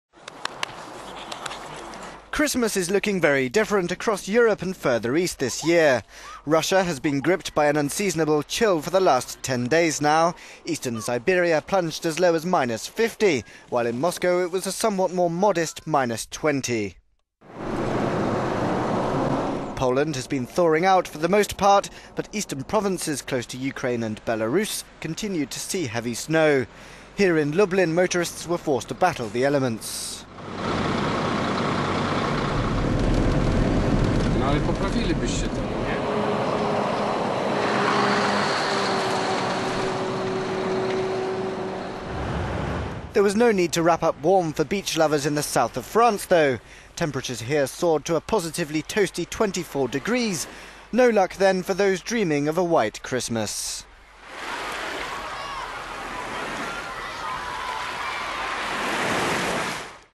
Weather extremes across Europe
Christmas is looking very different across Europe and further east this year.

Russia, which will actually celebrate Christmas day on January 7, has been gripped by an unseasonable chill for the last 10 days now.

Eastern Siberia plunged as low as -50°C, while in Moscow it was a more modest -20°C

Poland has been thawing out for the most part,
but eastern provinces close to Ukraine and Belarus continue to see heavy snow.

Germany and France have been seeing other extremes however, the southern parts of both countries experiencing temperatures in excess of 20°C.